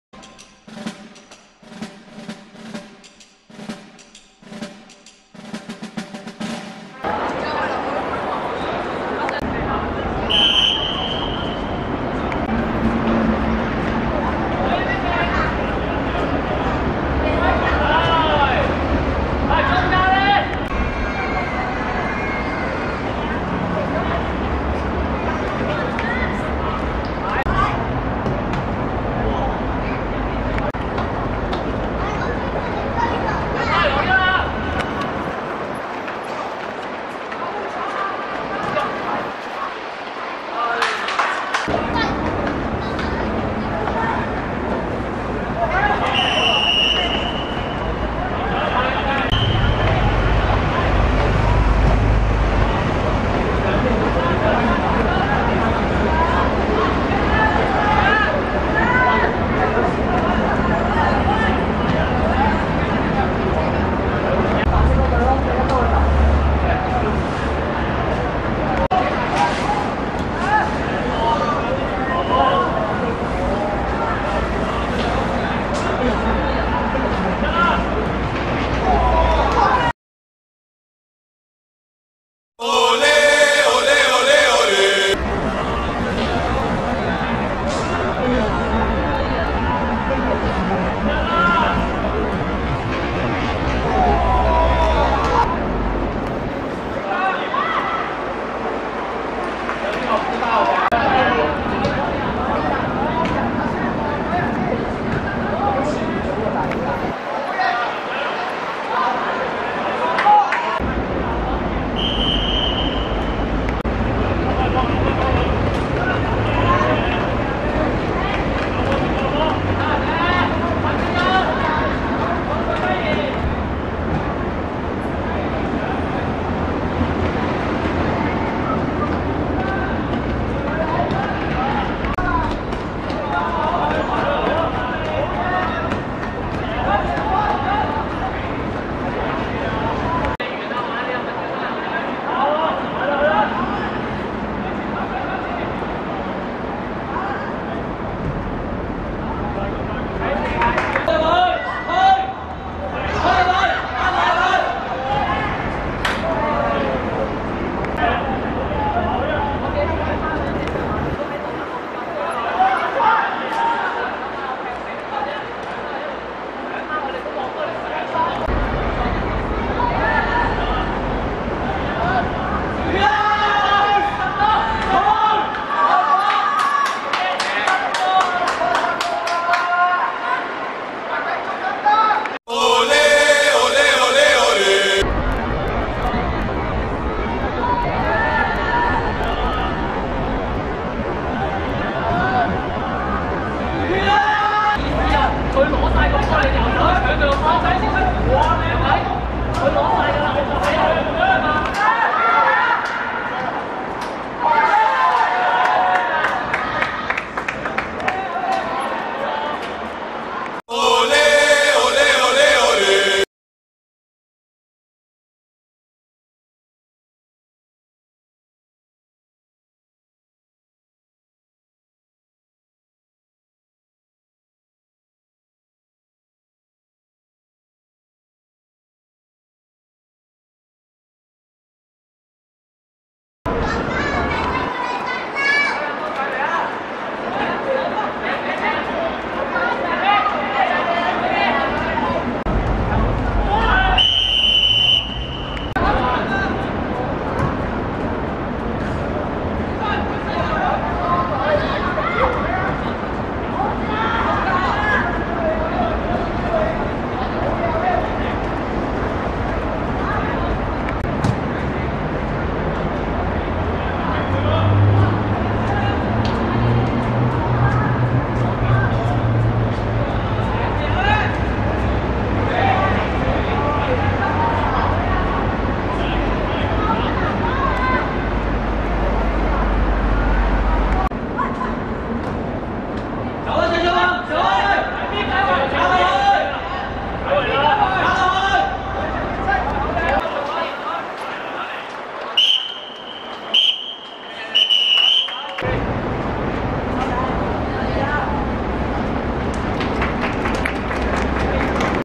白約翰vs鄧佩瓊(2017.3.9.元朗學界足球丙組)精華
2017.3.9.兆麟運動塲 18:30 白約翰(黃) 2 : 1 鄧佩瓊(白)